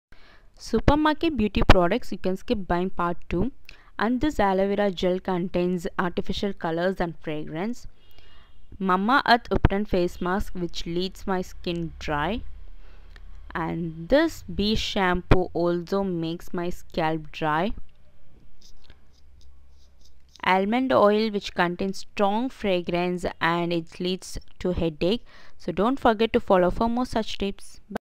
SUPER MARKET BEAUTY PRODUCTS YOU CAN SKIP PART-2
✨Supermarket Beauty Products You Can Skip Buying

🌿Guys here I showed some products which doesn’t suits my skin, everyone skin are different. Always do a patch test. Hope this video helped you❤️

❤️Save this post for later use
📍Don’t repost without credits
instagram for more useful updates